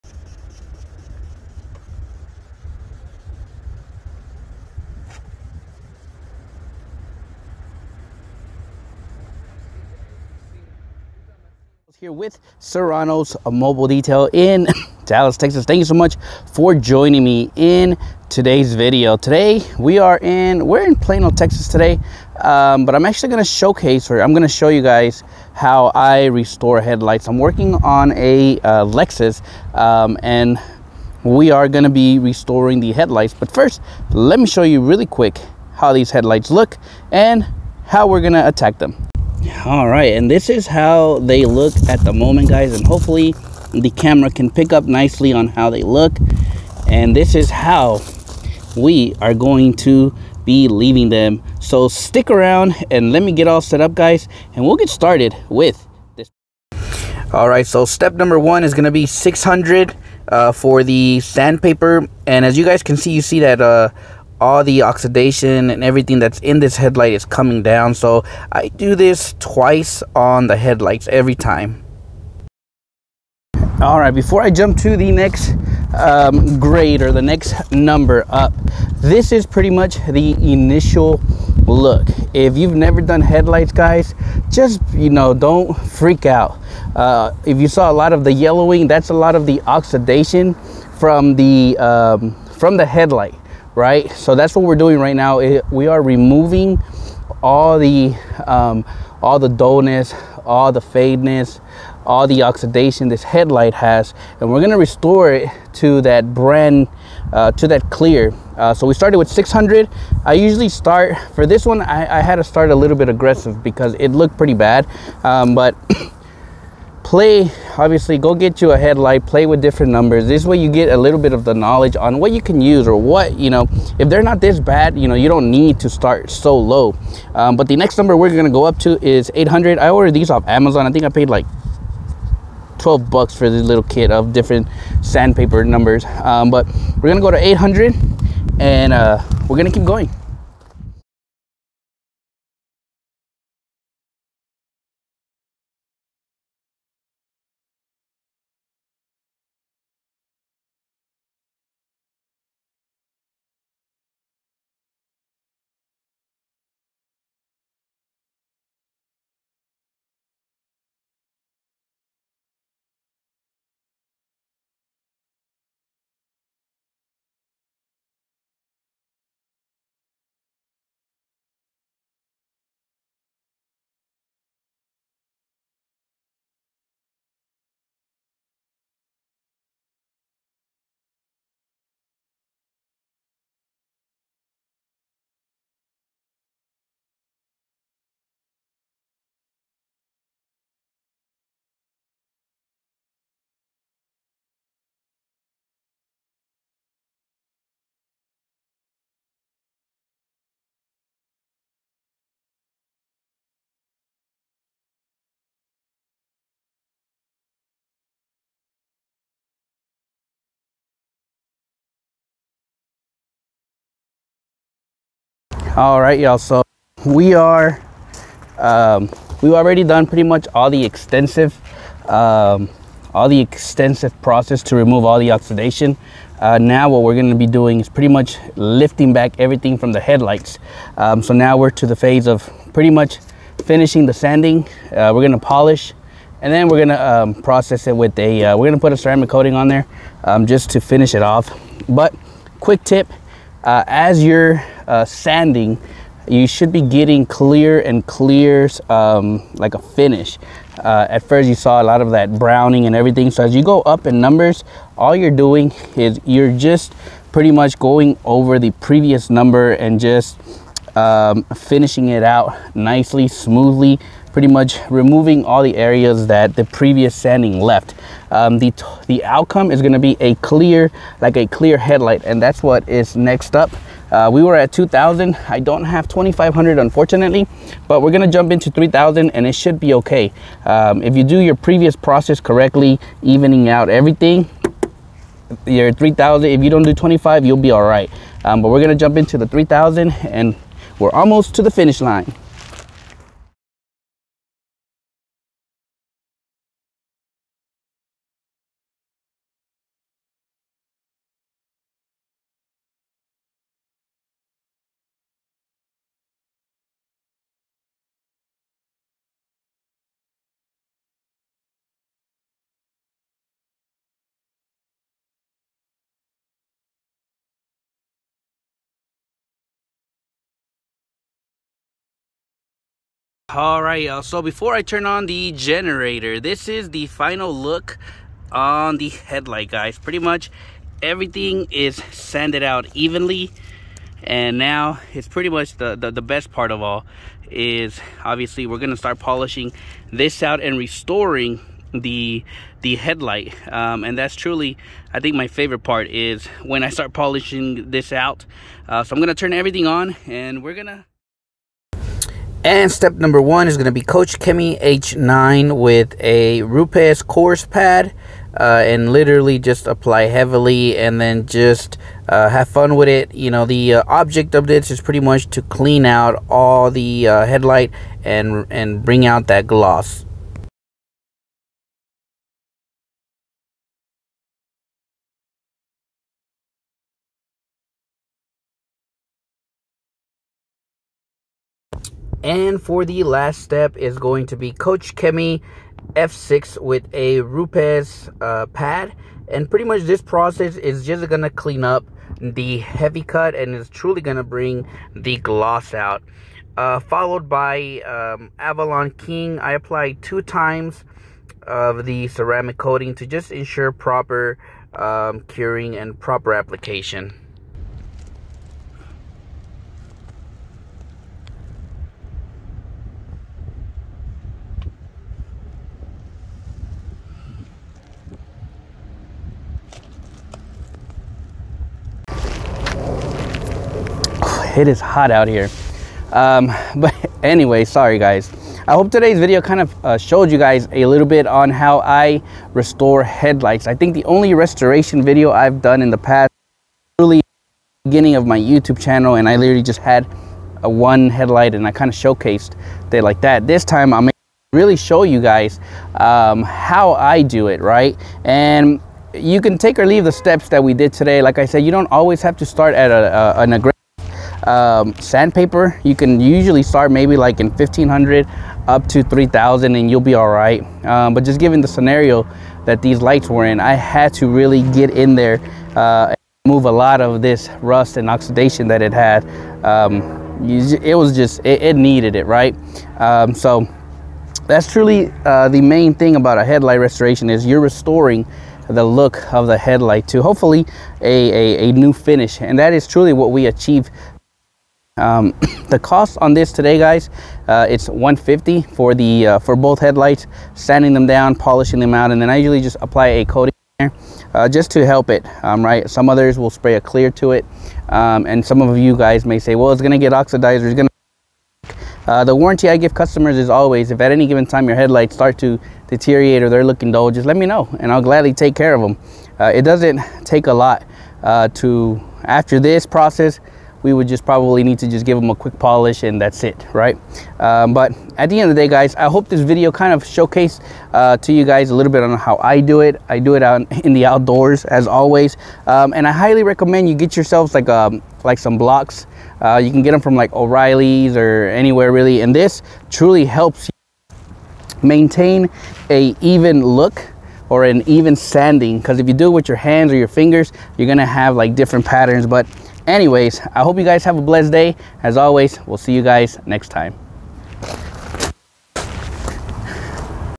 Headlight restoration LEXUS
welcome back, today I want to show you how I restore lights ,headlights